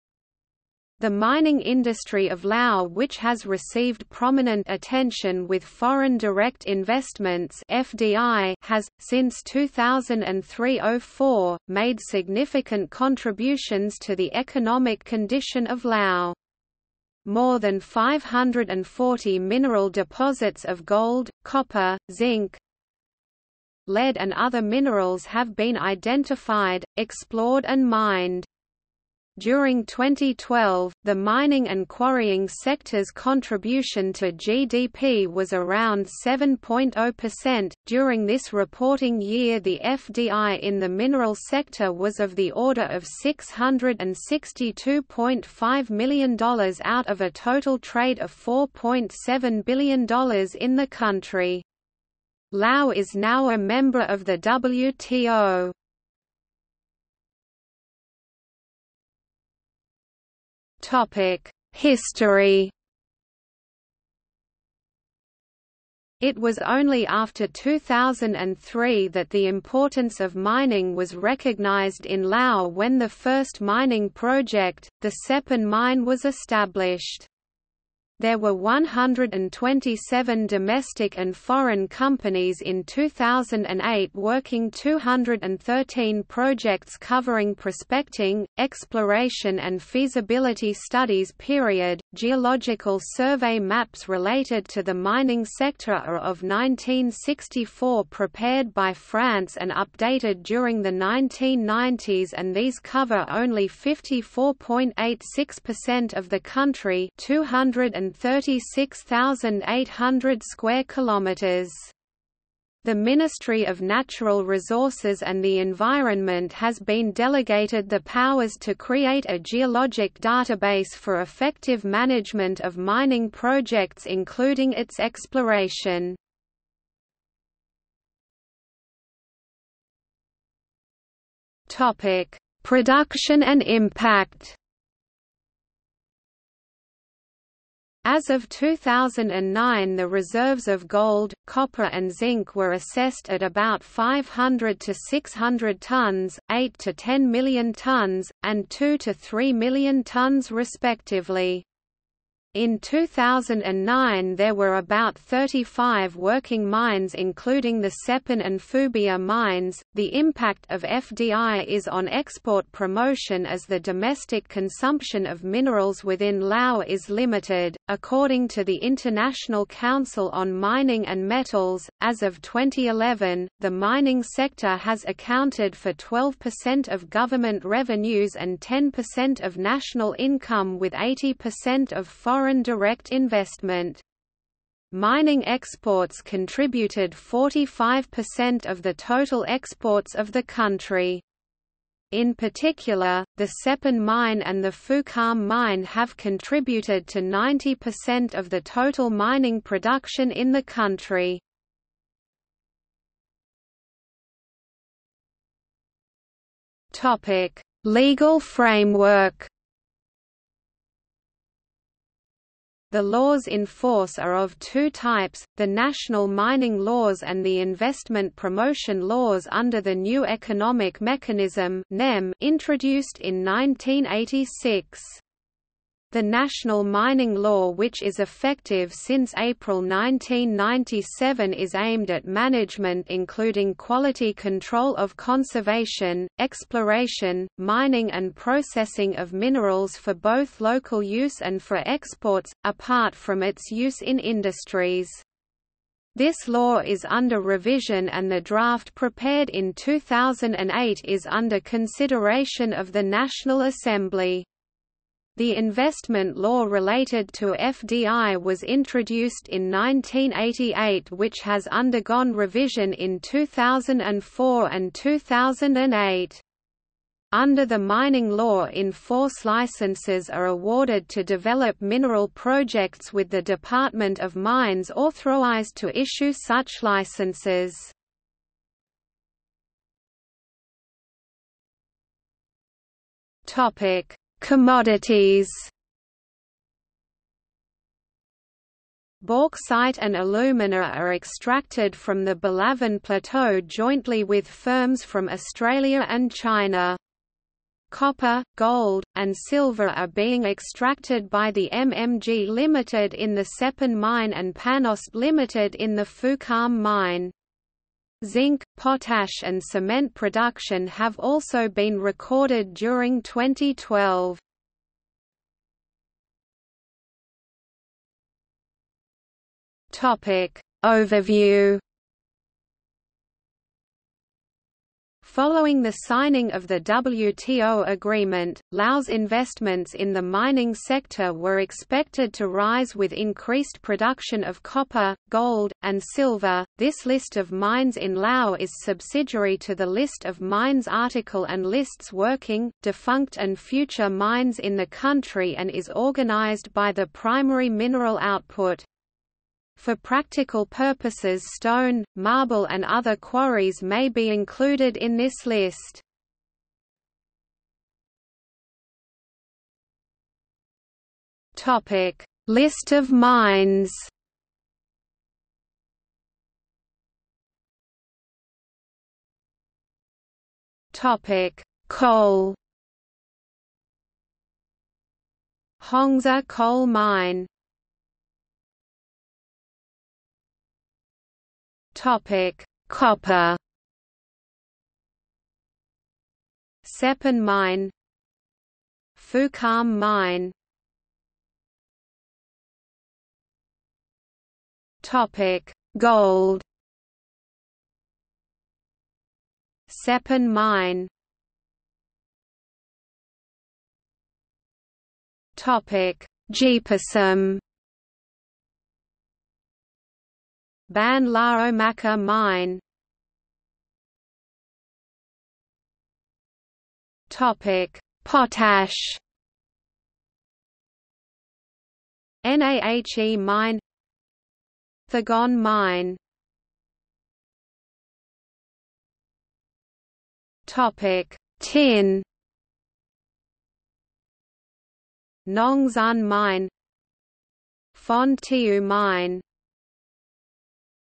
Mining industry of Laos | Wikipedia audio article
00:00:57 1 History
00:02:08 2 Production and impact
00:03:23 3 Legal framework
00:04:39 4 Commodities
00:05:16 5 Overview
00:05:58 6 List of mines
00:06:07 6.1 Coal
00:06:18 6.2 Copper
00:06:31 6.3 Gold
00:06:42 6.4 Gpysum
00:06:54 6.5 Potash
00:07:07 6.6 Tin

- Socrates

SUMMARY
The Mining industry of Laos which has received prominent attention with foreign direct investments (FDI) has, since 2003–04, made significant contributions to the economic condition of Laos. More than 540 mineral deposits of gold, copper, zinc,
lead and other minerals have been identified, explored and mined. During 2012, the mining and quarrying sector's contribution to GDP was around 7.0%; during this reporting year the FDI in the mineral sector was of the order of US$662.5 million out of a total trade of $4.7 billion in the country. Laos is now a member of the WTO.